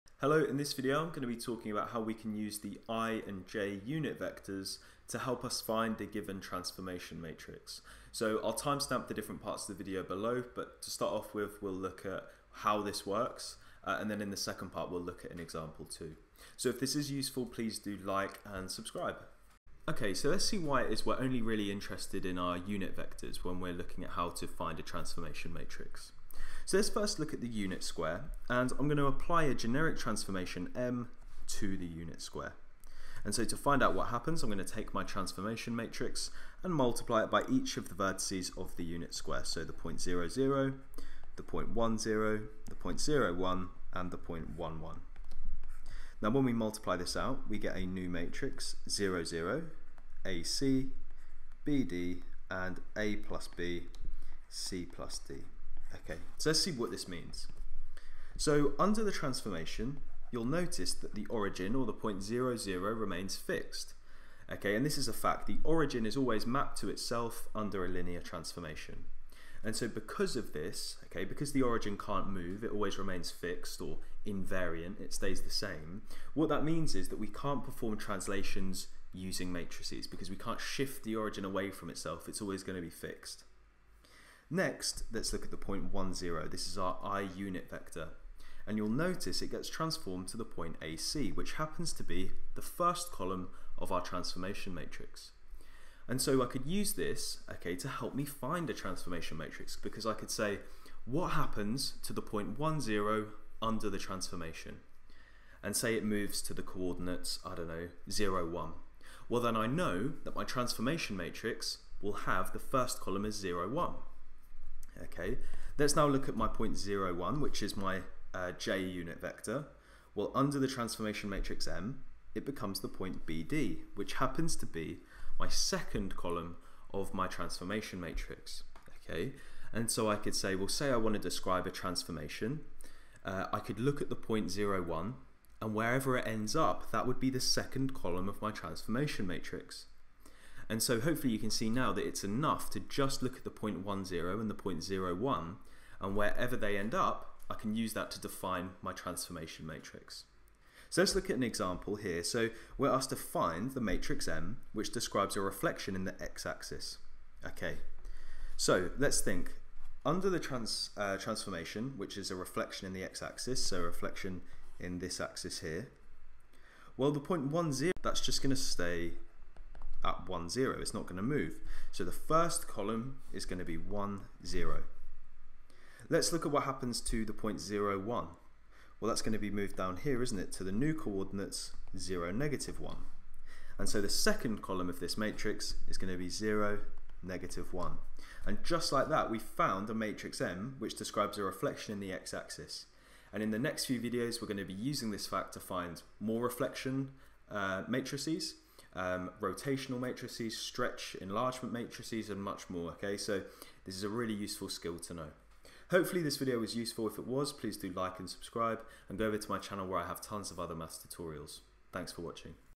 Finding a Transformation Matrix using i & j Unit Vectors (Matrices 10) | A-Level Further Maths
In this video I'll show you how to find a transformation matrix :)

Timestamps

Intro 0:00
Why we use unit vectors? 0:19
Example 2:30